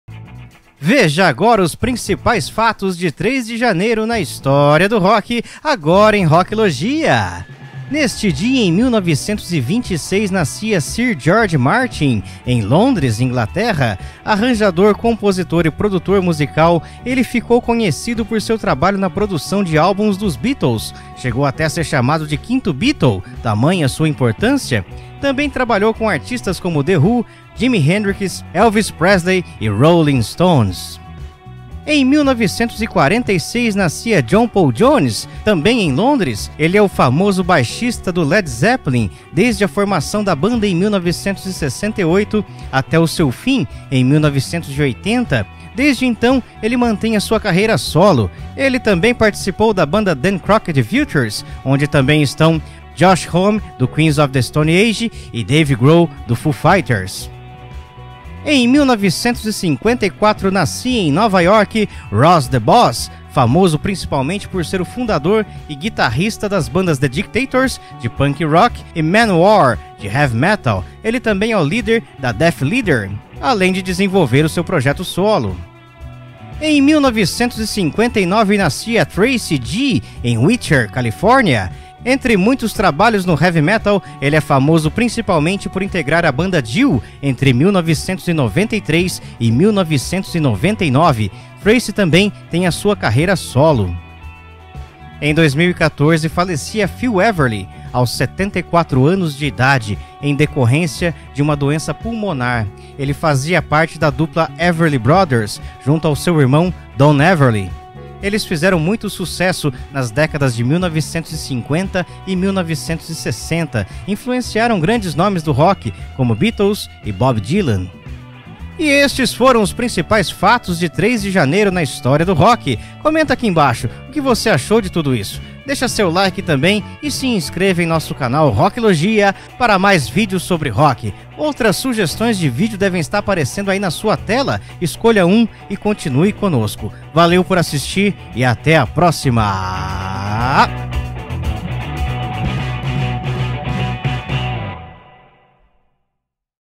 03 de Janeiro na História do Rock - ROCKLOGIA
Neste websódio de Rocklogia, os principais fatos de 03 de janeiro na História do Rock.

Neste dia, em 1926, nascia Sir George Martin, em Londres, Inglaterra. Arranjador, compositor e produtor musical, ele ficou conhecido por seu trabalho na produção de álbuns dos Beatles. Chegou até a ser chamado de Quinto Beatle tamanha sua importância. Também trabalhou com artistas como The Who, Jimi Hendrix, Elvis Presley e Rolling Stones.

Em 1946, nascia John Paul Jones, também em Londres. Ele é o famoso baixista e tecladista do Led Zeppelin desde a fundação da banda em 1968 até o seu fim em 1980. Desde então, mantém sua carreira solo. Ele também participou da banda  Them Crooked Vultures, onde também estão Josh Homme, do Queens of the Stone Age e Dave Grohl, do Foo Fighters.

Em 1954, nascia em Nova York, Ross the Boss, famoso principalmente por ser o fundador e guitarrista das bandas The Dictators, de punk rock, e Manowar, de Heavy Metal. Ele também é líder da Death Leader, além de desenvolver seu projeto solo.

Em 1959, nascia Tracy G, em Whittier, California. Entre muitos trabalhos no Heavy Metal, ele é famoso principalmente por integrar a banda Dio, entre 1993 e 1999. Tracy também tem sua carreira solo.

Em 2014, falecia Phil Everly, aos 74 anos de idade, em decorrência de uma doença pulmonar. Ele fazia parte da dupla Everly Brothers junto com seu irmão Don Everly. Eles fizeram muito sucesso nas décadas de 1950 e 1960, influenciaram grandes nomes do Rock como Beatles e Bob Dylan.

Capítulos:
00:00 - Introdução
00:07 - Nascia o produtor George Martin
00:31 - Nascia John Paul Jones, baixista do Led Zeppelin
00:58 - Nascia Ross The Boos, guitarrista do The Dictators e Manowar
01:16 - Nascia Tracy G., guitarrista da Dio
01:34 - Falecia Phil Everly, dos Everly Brothers